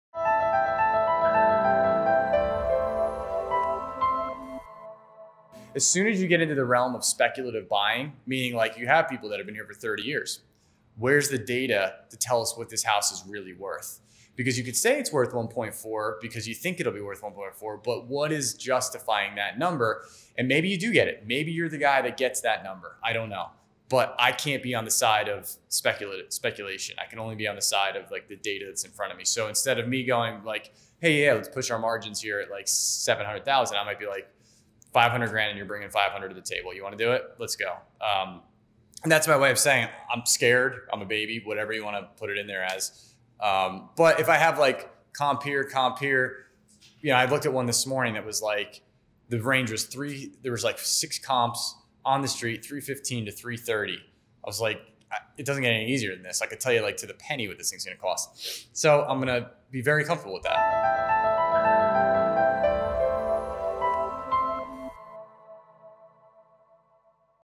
KW Philadelphia Presentation - Speculative Buying Is Dangerous
Speculative buying is one of the highest risk/reward games in real estate investing.  Most experienced investors tend to not be speculative in nature because the downside to being wrong on a speculative buy is very penalizing.  The upside of a speculative buy can be very rewarding but can also create a bubble of false confidence.  Experienced investors tend to like to invest in areas where they are certain of the exit price on their project rather than guessing in a speculative scenario.